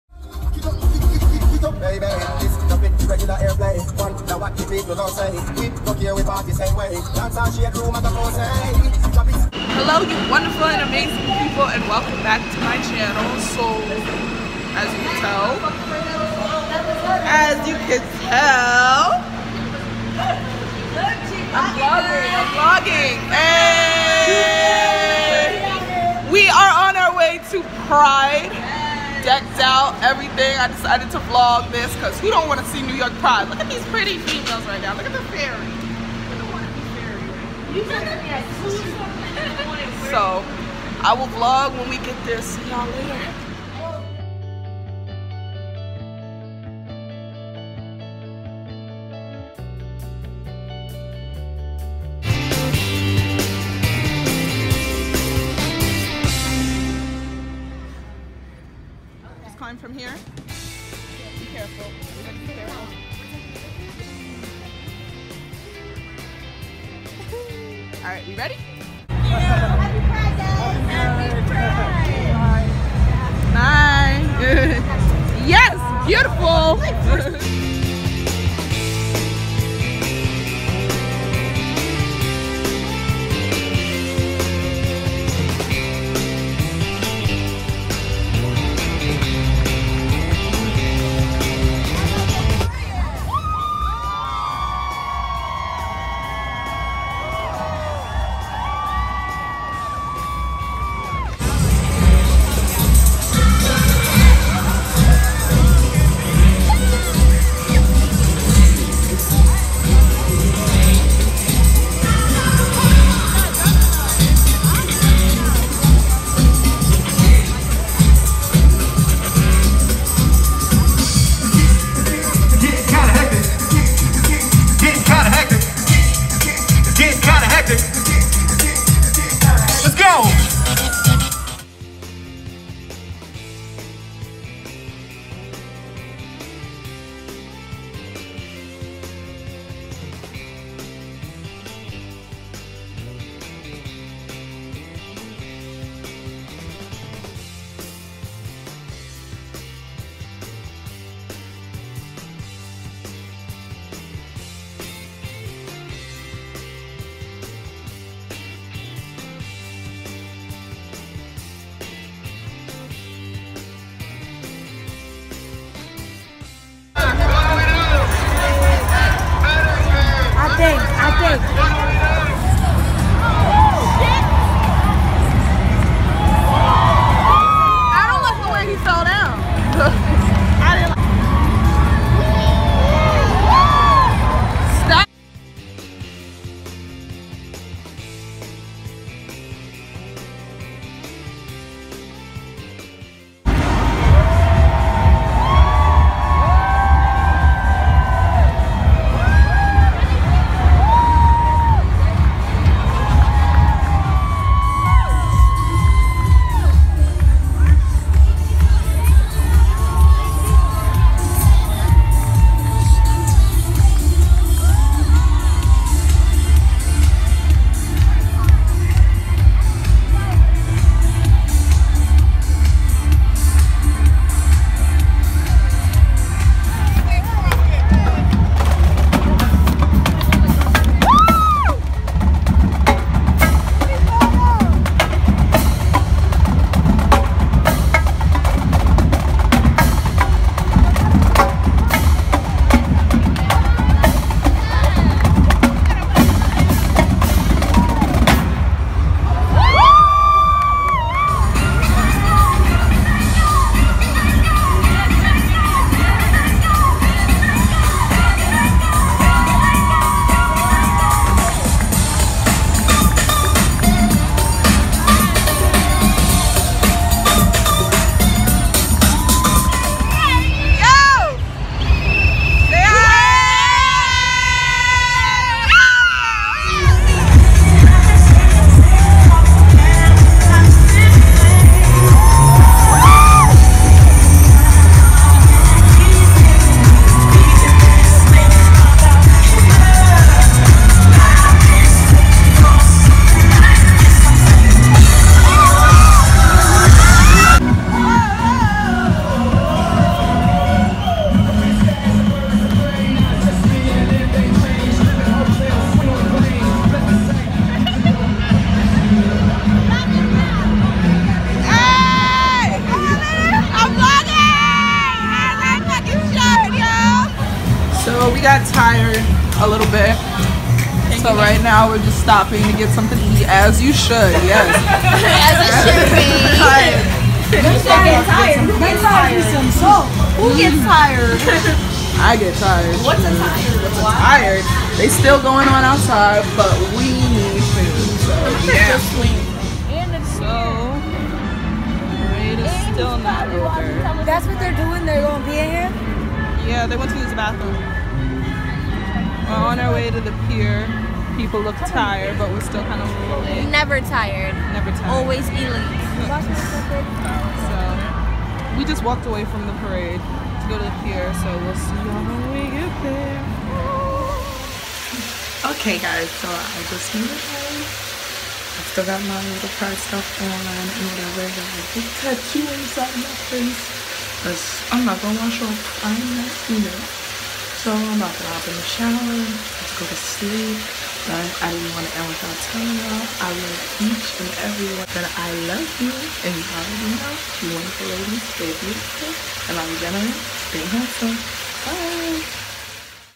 Everything is so colorful 🏳️‍🌈🏳️‍🌈
Hello you wonderful and amazing people and welcome back to my channel so yesterday I went to New York pride and it was amazing I was able to capture a little bit to share with everyone on my channel so hope your enjoy.